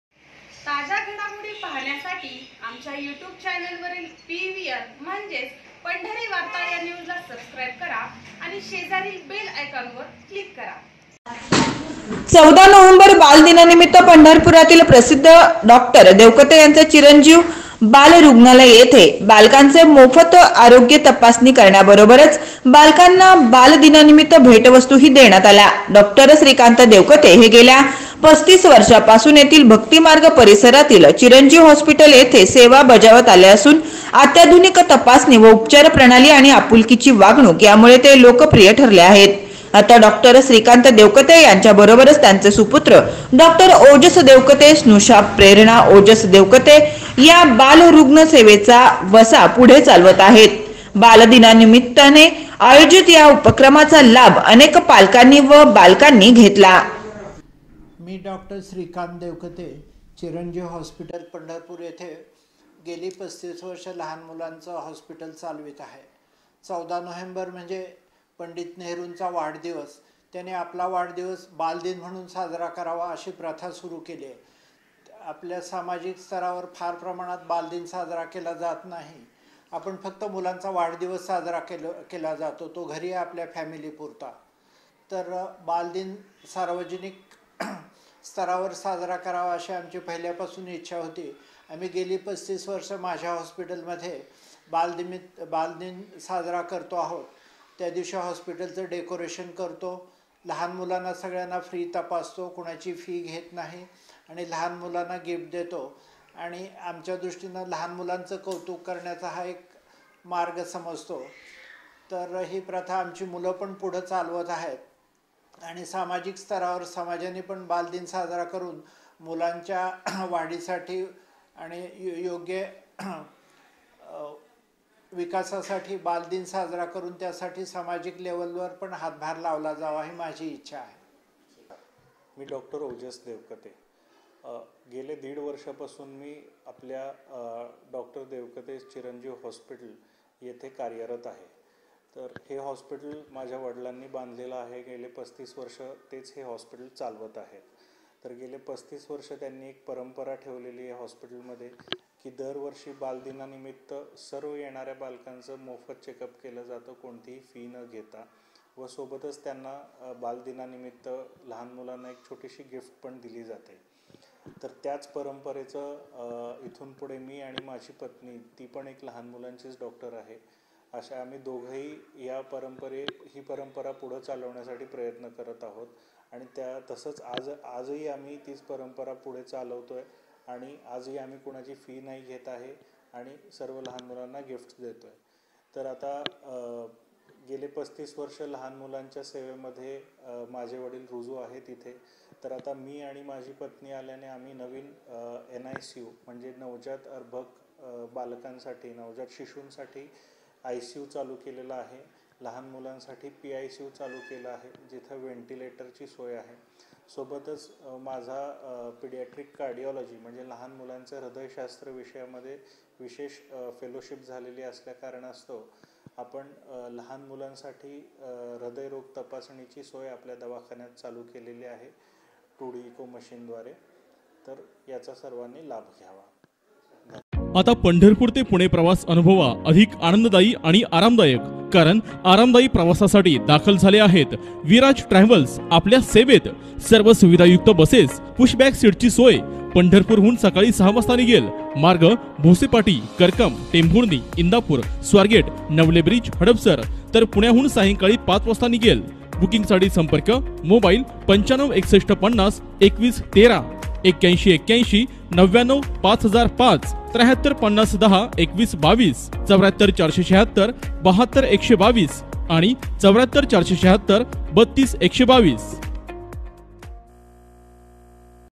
डॉ.देवकते यांच्या चिरंजीव हॉस्पिटल येथे बालदिन उत्साहात साजरा
बालकांची मोफत आरोग्य तपासणी व बालकांना देण्यात आले '